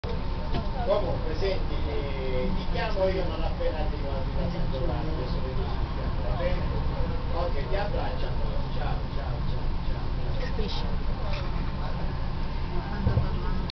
In a train, in a boat, on the water...
This is what you see when you cross from Messina to Regio di Calabria in a train.  Quite spooky because you can't get out of your train car.